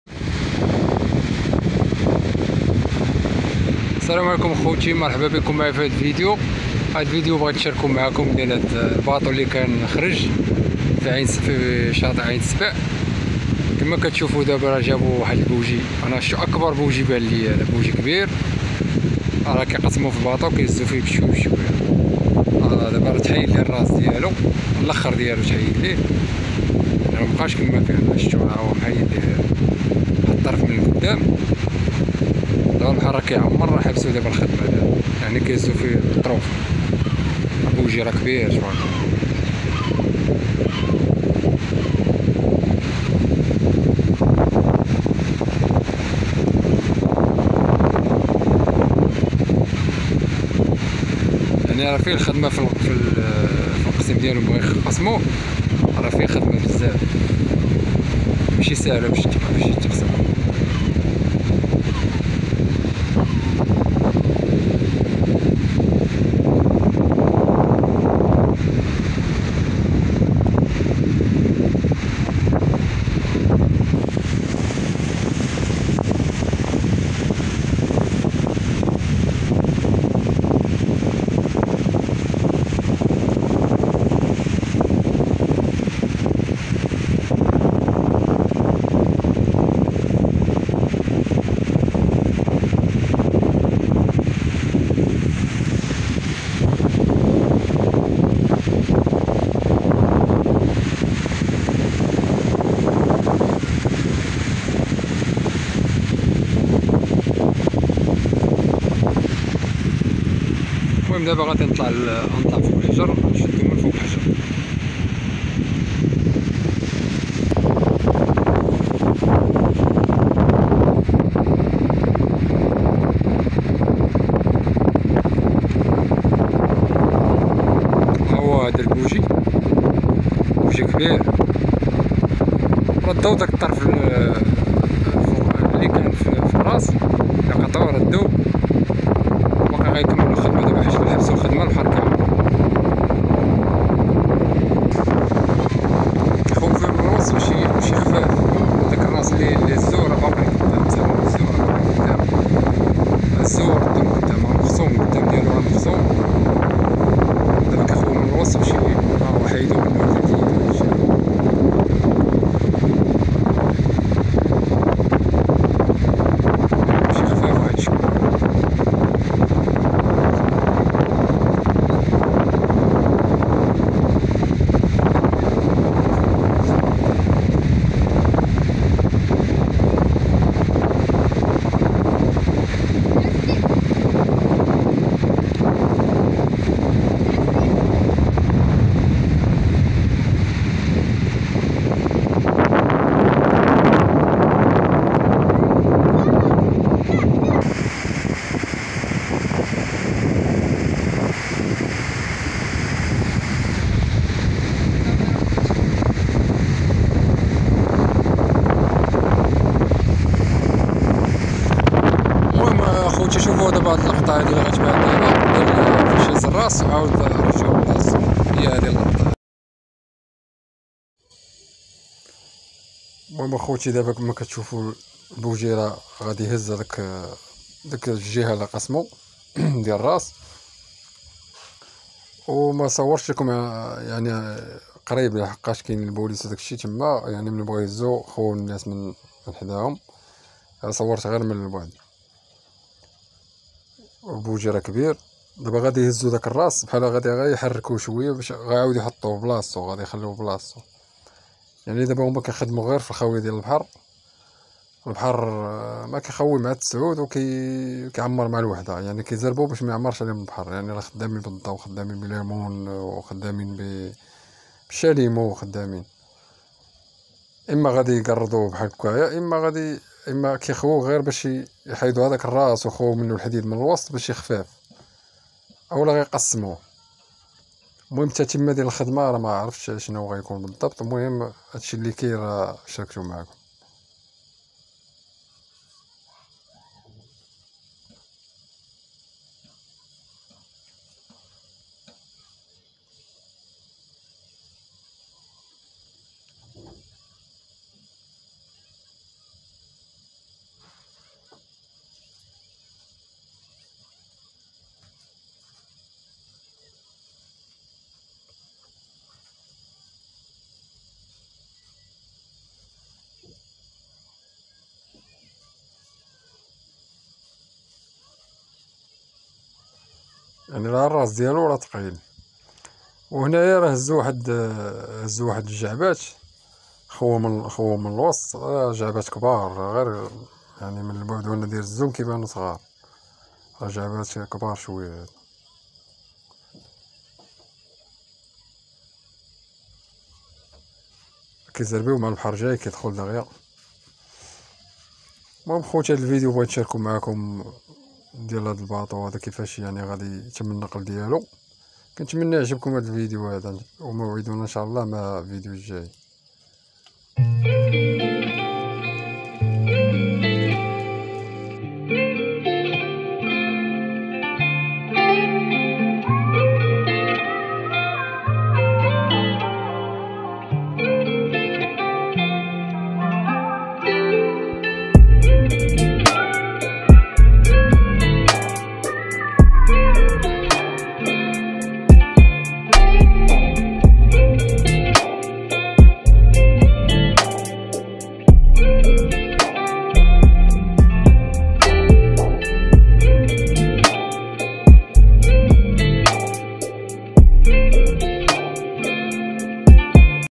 أجي تشوف👈 الطريقة# لي غادي اتم بيها نقل الباطو#🛳️لي خرج في بحر#🌊الراحة عين السبع
مرحباً بكم في قناة طعمة مغربي حر المرجو الإشتراك في القناة ليصلكم كل جديد 🔔 وشكراً